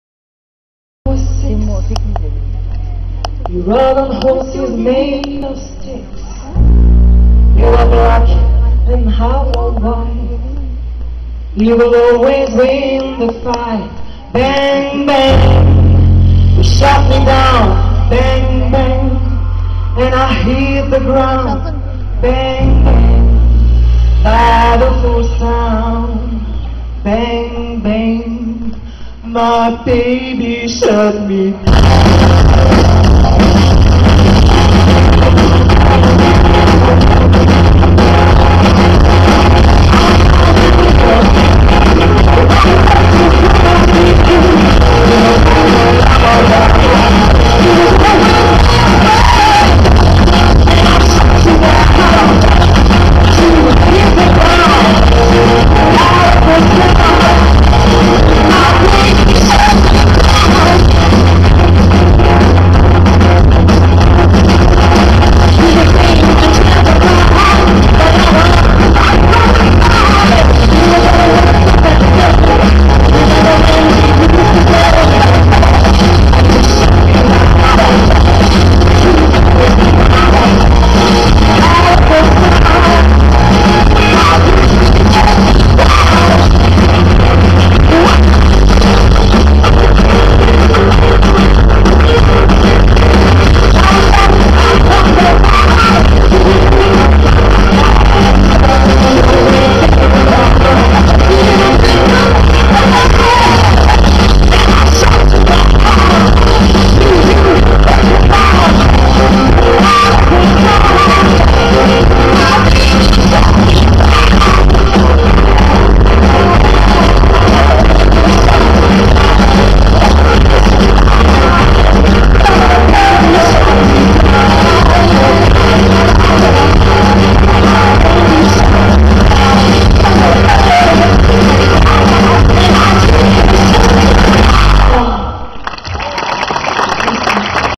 Première partie - Concert Gérald De Palmas - Mimizan 2012
Un groupe joue la première partie du concert de Gérald de Palmas, voici un morceau!